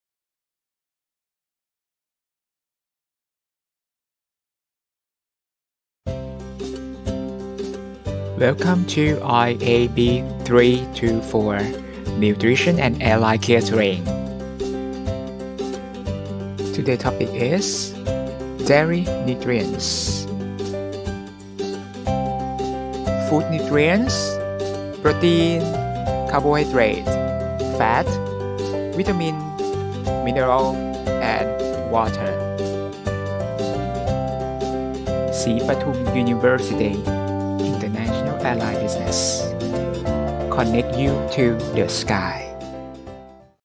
IAB324 Week3 Topic Introduction
IAB324 Nutrition and Airline Catering
Week3 Topic Introduction
by Assistant Professor Dr.Vasavat Sutinyamanee
Head of International Airline Business Department
Sripatum University, International College